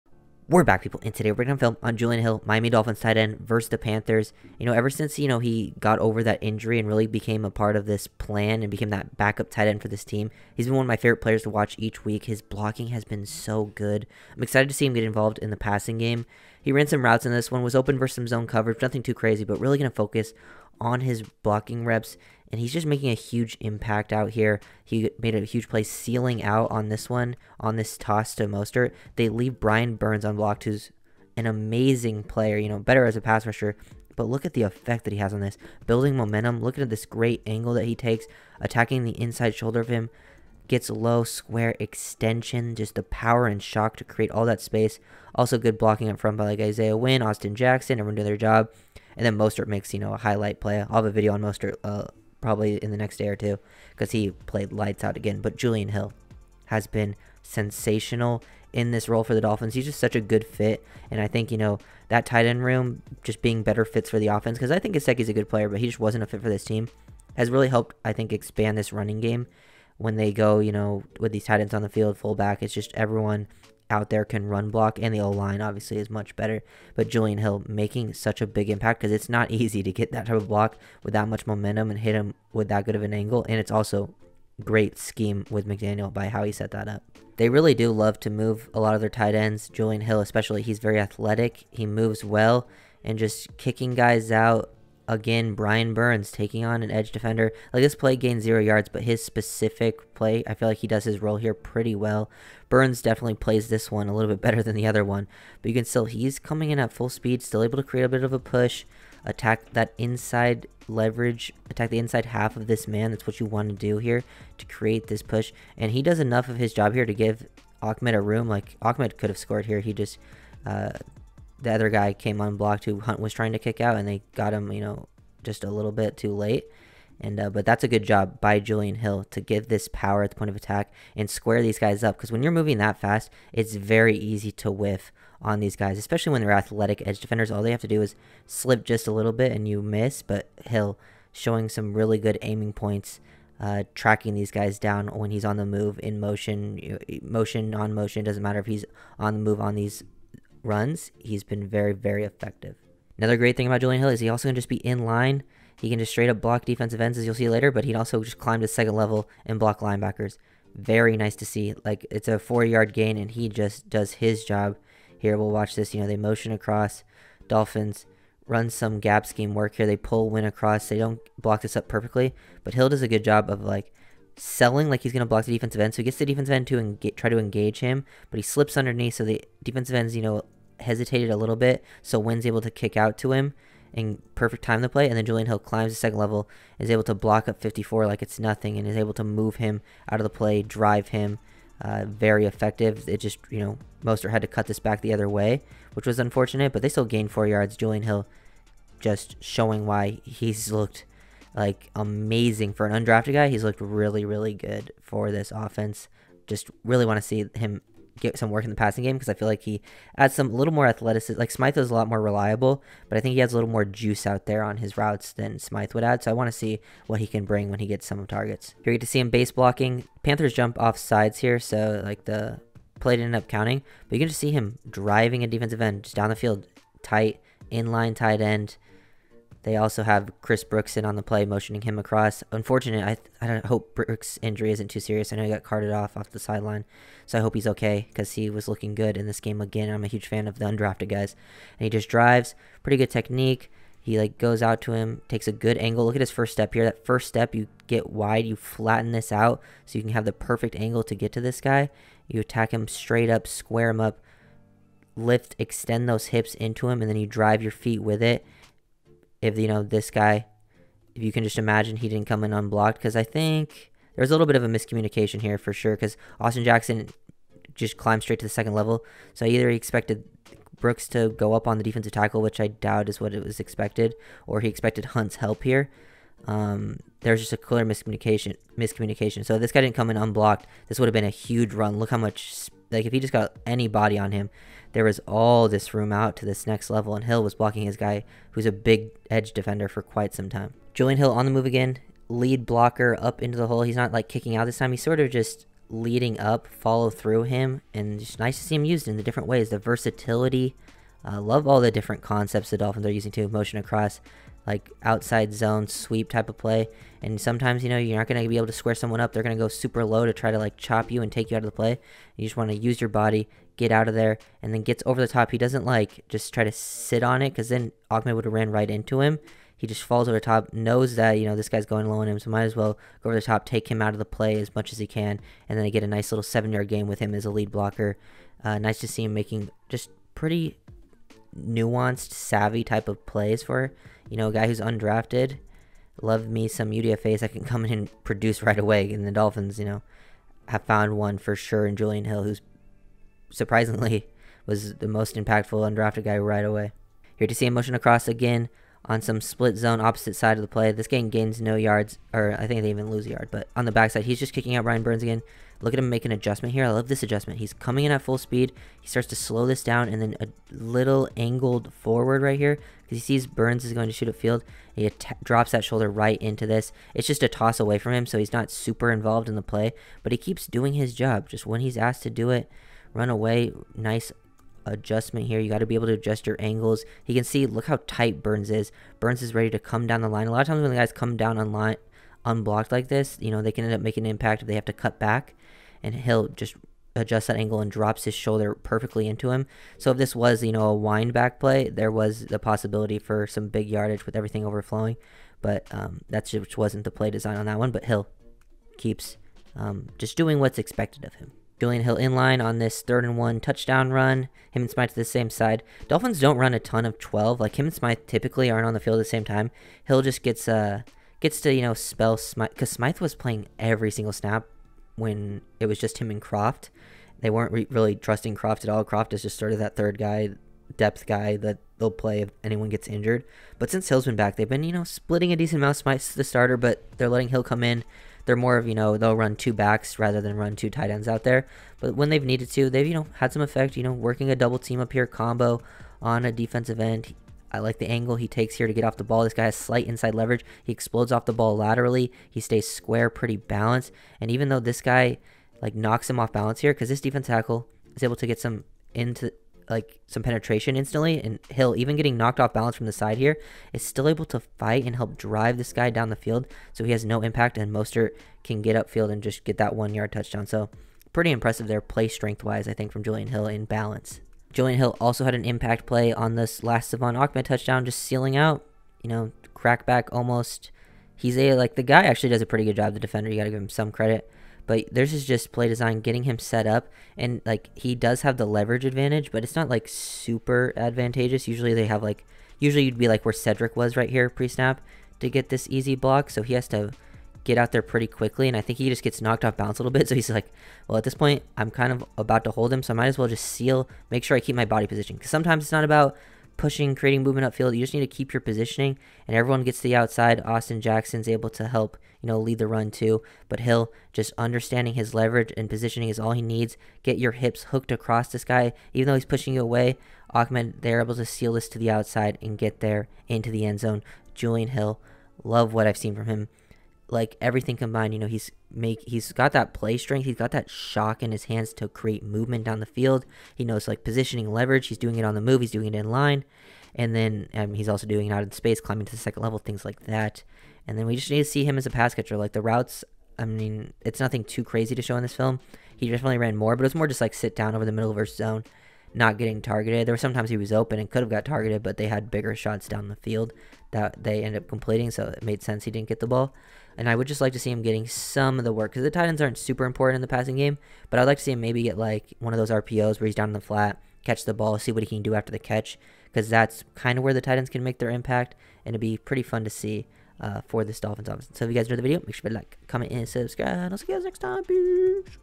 Film Breakdown: Julian Hill put on a Run Blocking Clinic vs Panthers
Julian Hill Miami Dolphins TE